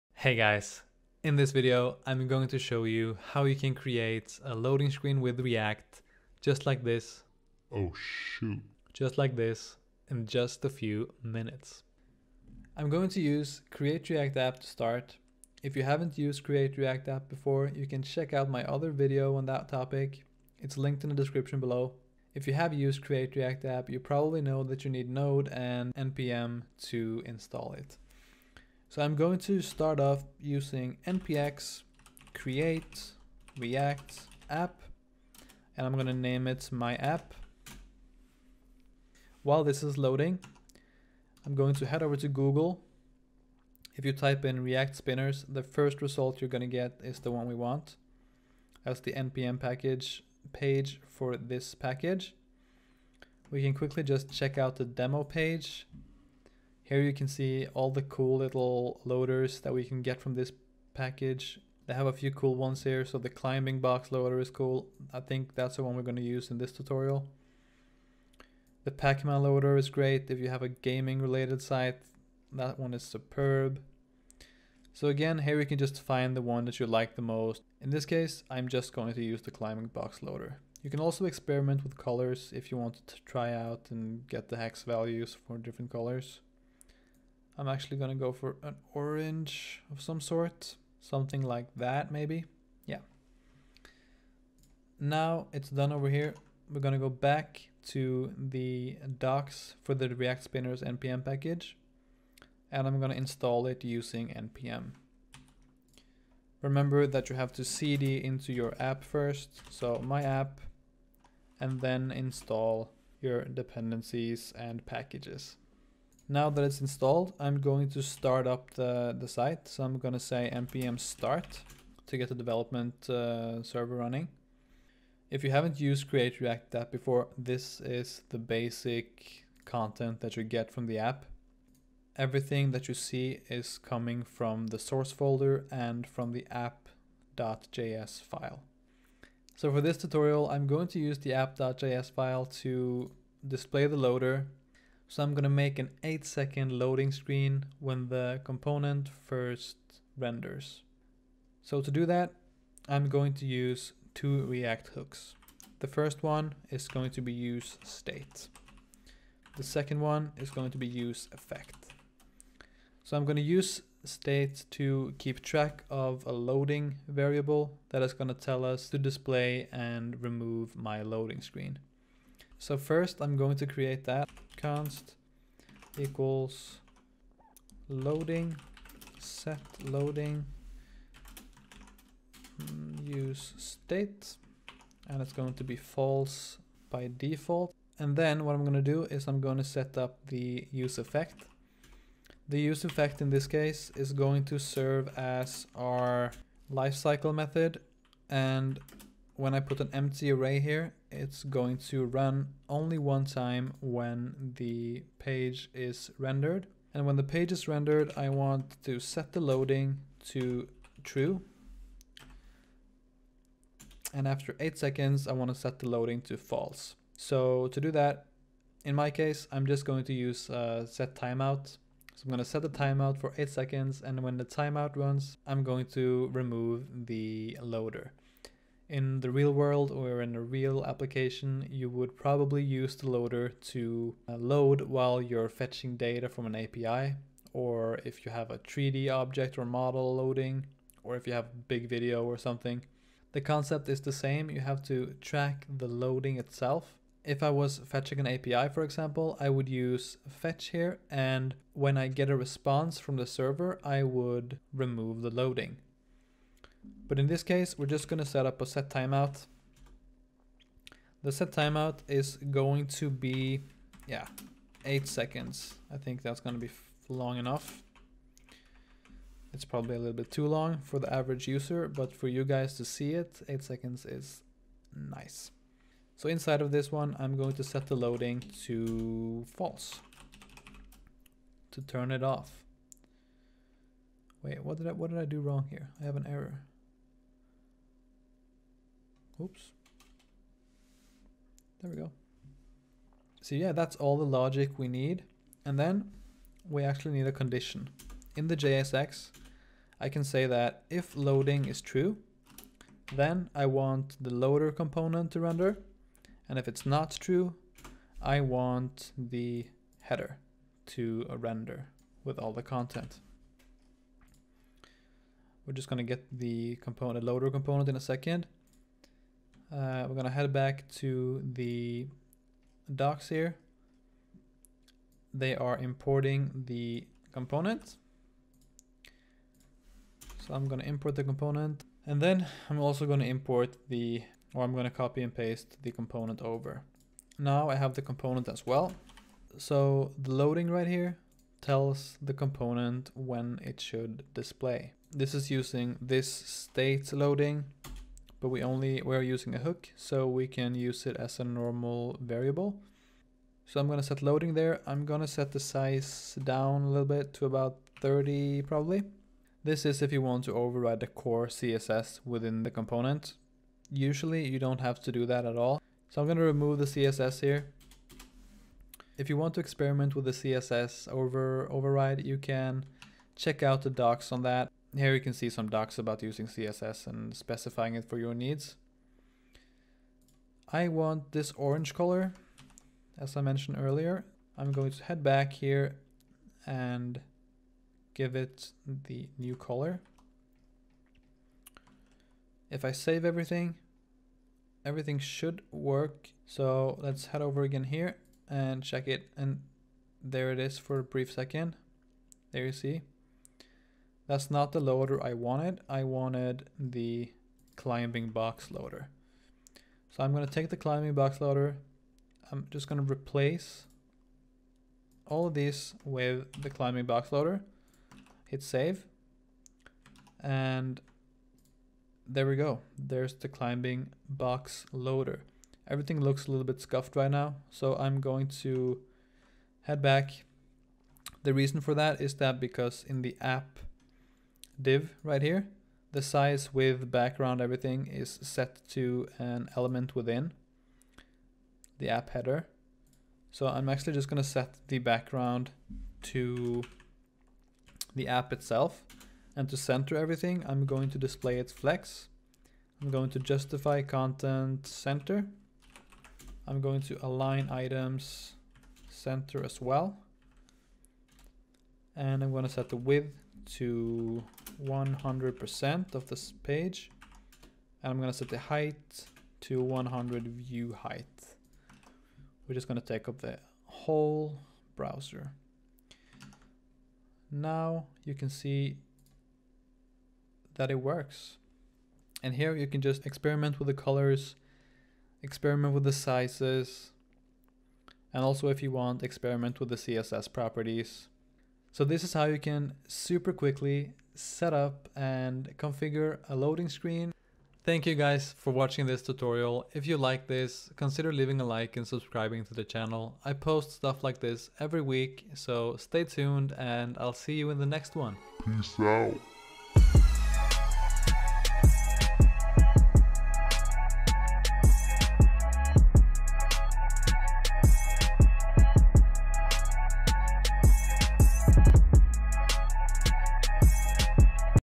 Loading Screen with React Tutorial | Beginners (2021)
In todays video we will be looking at how to create a simple loading screen with react.js. We are using the react-spinners package with predefined components. Loading with react in under 10 minutes!